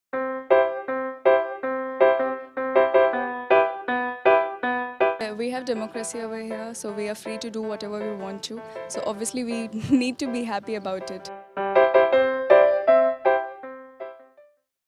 Youth of India celebrate democracy this Independence Day
Youth from around India come and share their reasons to smile this Independence day on Smilesofindia.com - the happy stories platform powered by Himalaya Sparkling White Toothpaste.